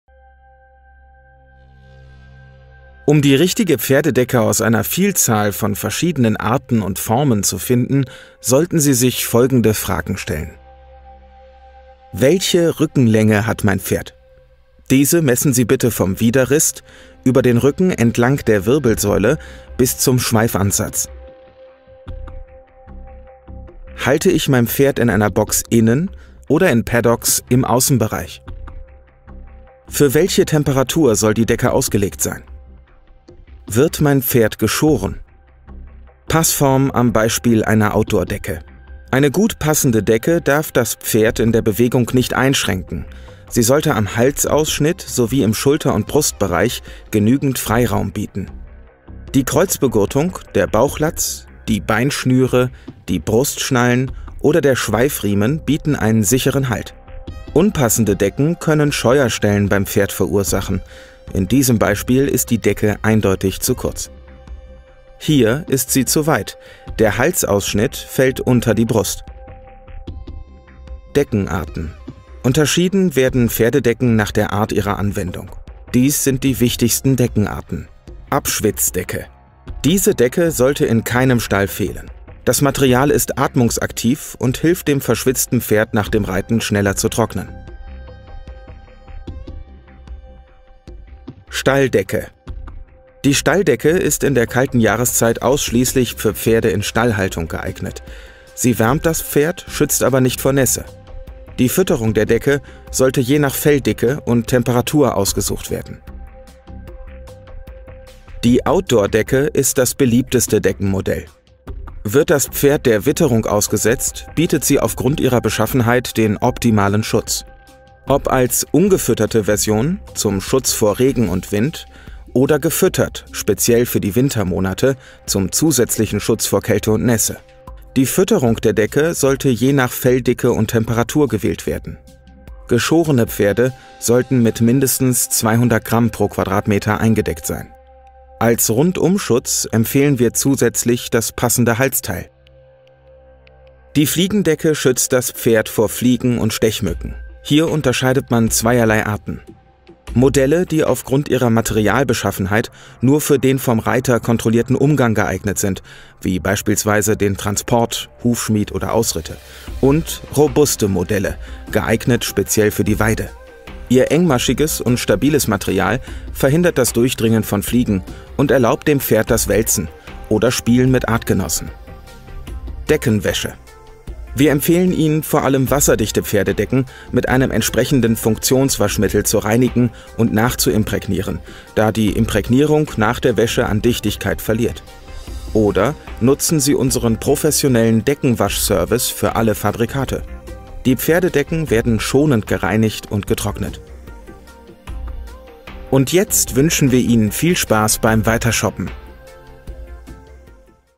Pferdedecken -- Krämer Pferdesport
Rückenlänge des Pferdes richtig messen. Pferdedecke für den Innen- oder Außenbereich? Passform und Deckenarten: Abschwitzdecke, Stalldecke, Outdoordecke oder Fliegendecke? Pferdedecken richtig waschen und pflegen.